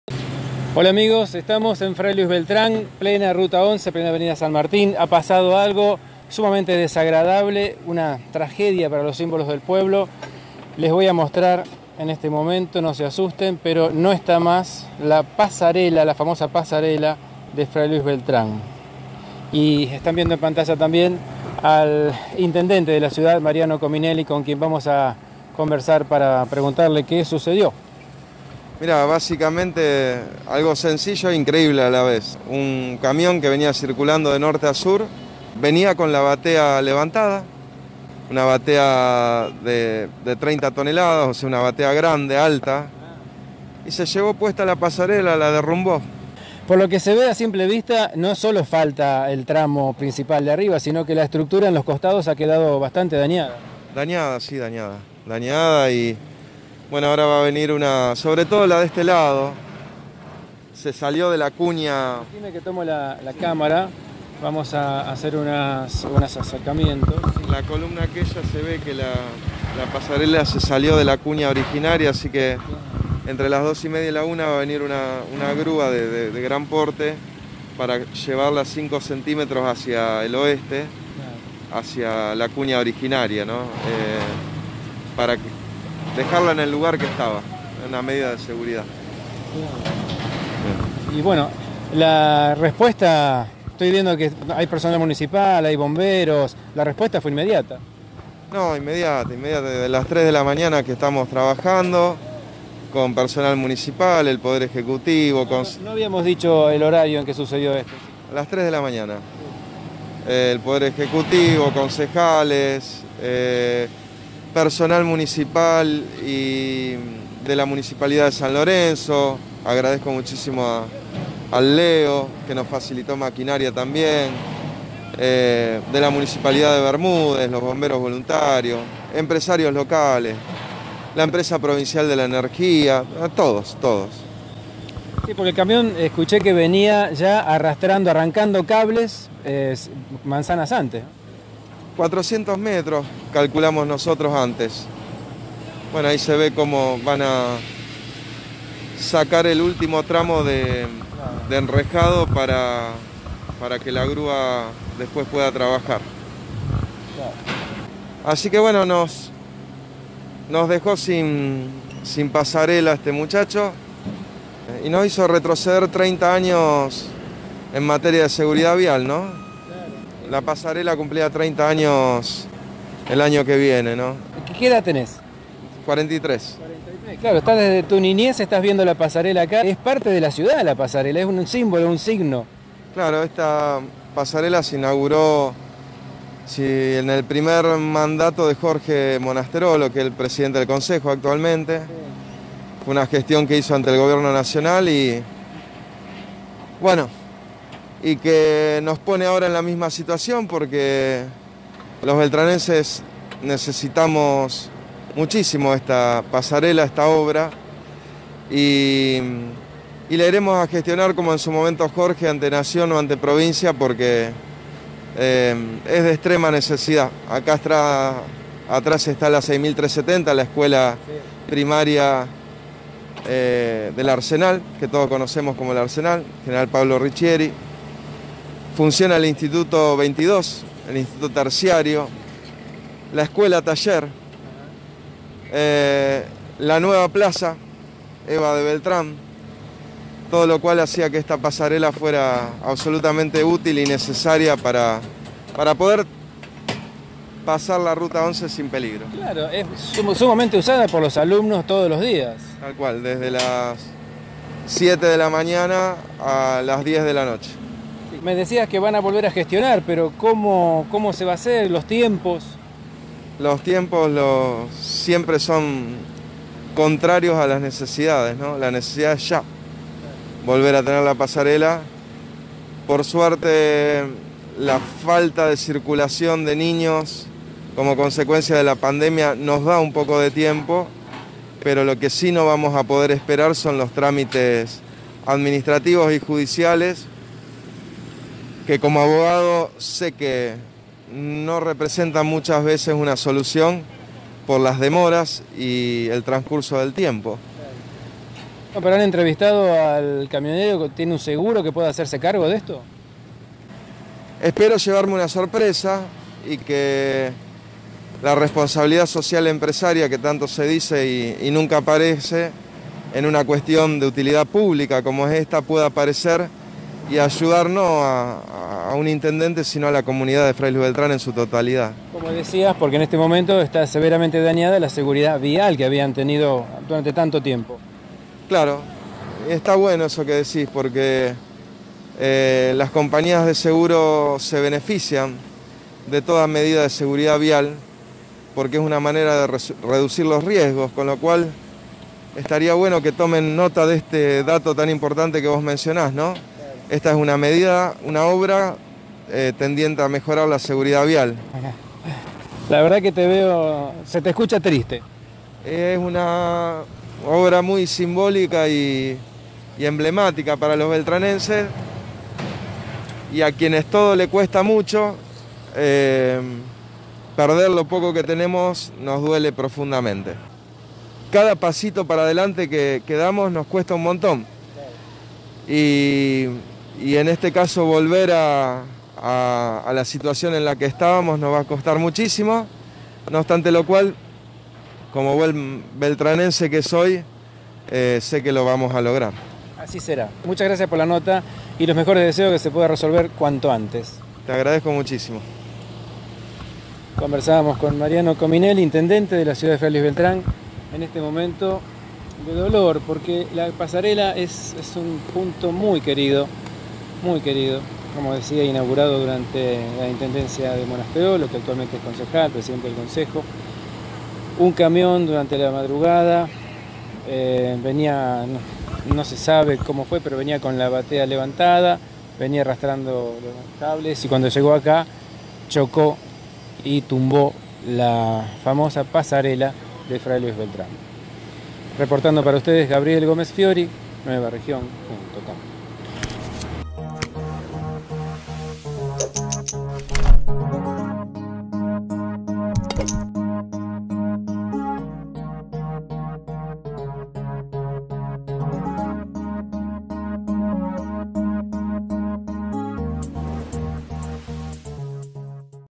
CON INTENDENTE MARIANO COMINELLI POR PASARELA CAIDA
CON INTENDENTE MARIANO COMINELLI
UN CAMIÓN DERRUMBÓ LA FAMOSA PASARELA DE FRAY LUIS BELTRÁN

Sucedió a las 3 de la mañana del mismo Miércoles 10 de Junio.
Inmediatamente acudieron empleados municipales, bomberos, y la ayuda de otras ciudades vecinas.

La pasarella fue nombrada "Pedro Cerliani" por aquél querido intendente. Cumpliría 30 años en 2021. Fue inaugurada durante la primera intendencia de Jorge Monasterolo, actualmente Presidente del Cncejo Municipal.

Comienza ahora una urgente gestión para restaurarla o, quizá, hacer una nueva más amplia todavía pasando tanto la ruta como las vías. ¿Por qué no?

Filmado en  FRAY LUIS BELTRÁN
provincia de Santa Fe, Argentina, 10 JUNIO 2020

para NUEVAREGION.COM